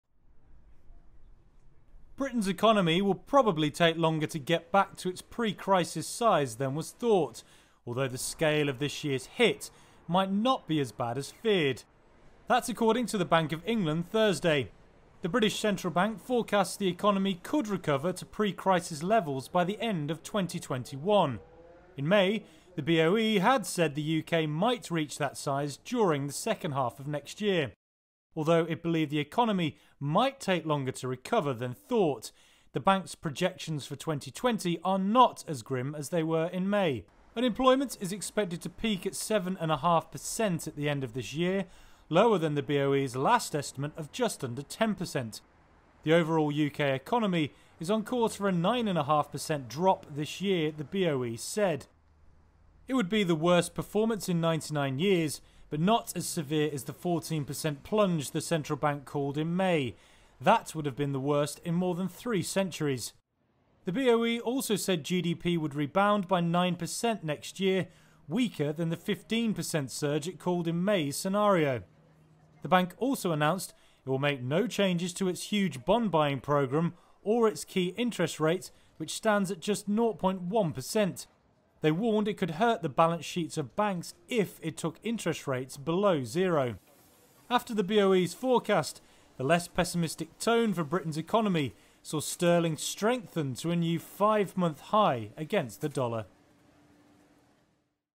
Bank of England sees slower economic recovery
The Bank of England said on Thursday that Britain's economy would probably take longer to get back to its pre-pandemic size than it previously thought, even if the scale of the hit this year might not be quite as bad.